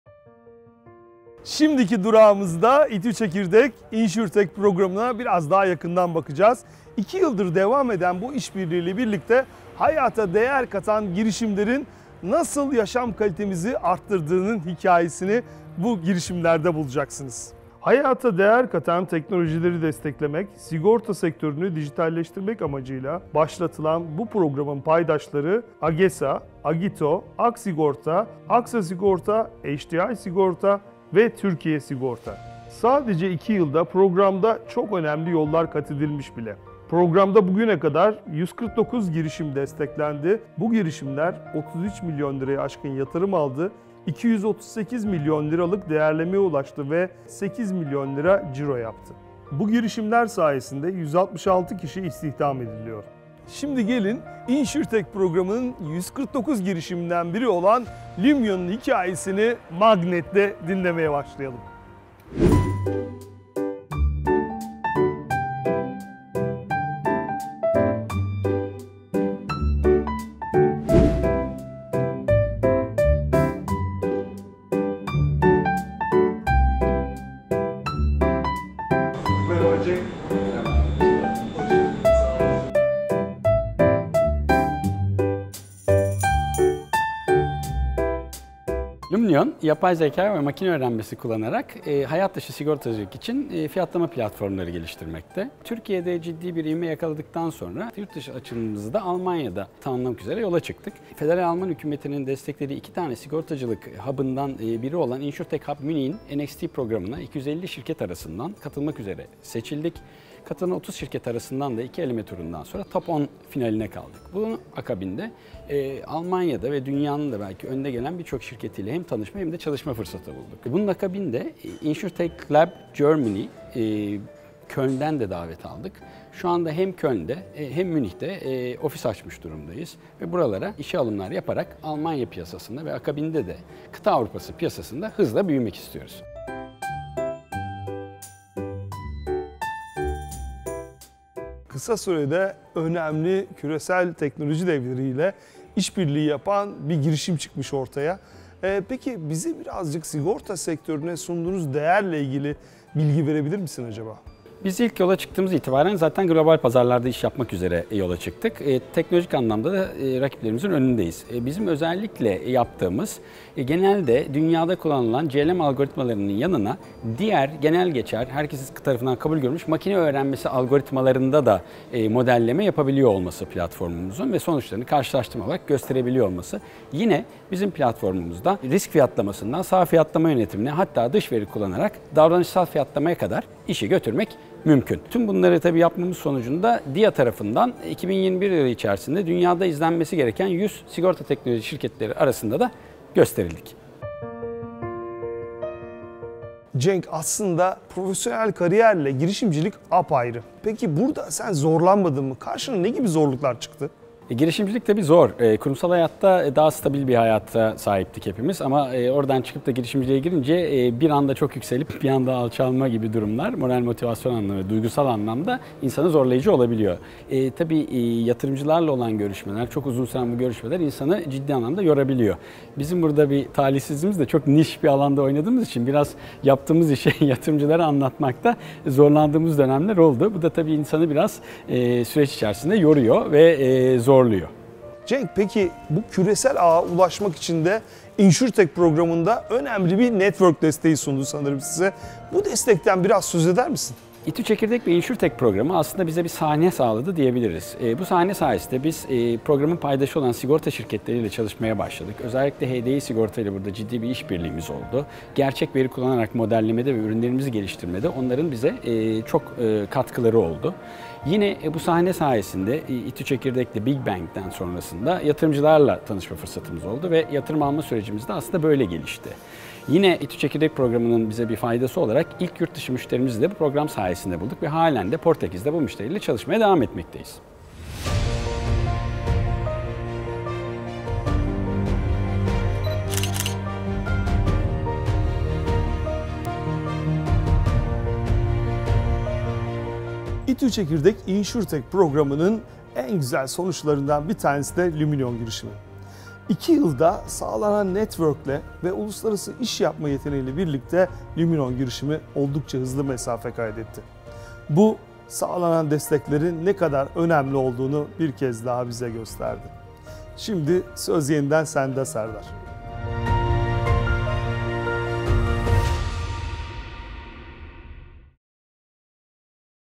İTÜ Çekirdekli Lumnion'un Hikayesi, Big Bang'de!
İTÜ Çekirdek girişimlerimizden olan, İTÜ Magnetli firma olarak olarak çalışmalarına devam eden ve InsurTech Programı kapsamında AgeSA, Agito, AKSigorta, Axa Sigorta, HDI Sigorta ve Türkiye Sigorta  ve Türkiye Hayat Emeklilik tarafından desteklenen Lumnion'un hikayesini Kurucu Ortak ve CEO'su Cenk Tabakoğlu'ndan dinliyoruz.